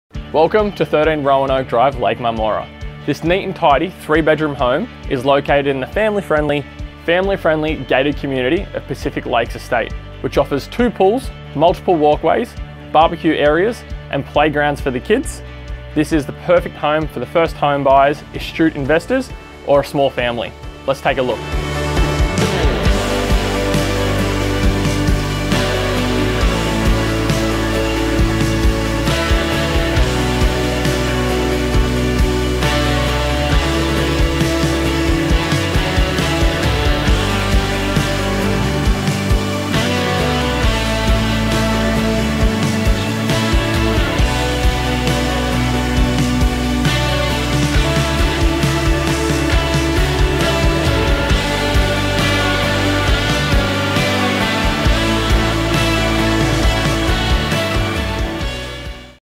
13 Roanoke Lake Munmorah
This property is located in a quiet street and backs onto reserve. 3 bedrooms - built-ins to all, air conditioning, spacious lounge room, separate dining room off modern kitchen with dishwasher, ceiling fans to main bedroom and living areas, separate toilet. Covered front porch and a covered entertaining area at rear, over-sized single garage with drive-through access to extra undercover spot at rear, easy care yard/gardens, garden shed. Access to the estates swimming pool/BBQ areas/playground and paved walking paths. All this and more within a short drive to beaches & national park.